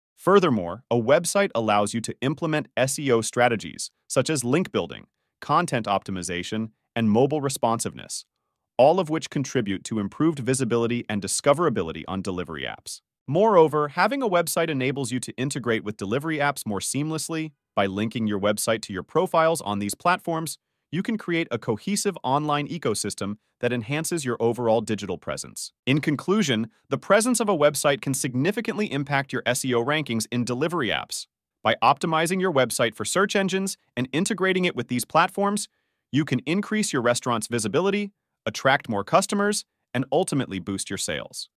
Unveiling the Impact: Website Presence on SEO Rankings in Delivery Apps. Part 3.
Unlock the secrets behind boosting your restaurant's visibility on food delivery apps! Join us as we explore the impact of having a website on SEO rankings in delivery apps. Discover how optimization techniques can enhance your restaurant's online presence and attract more customers.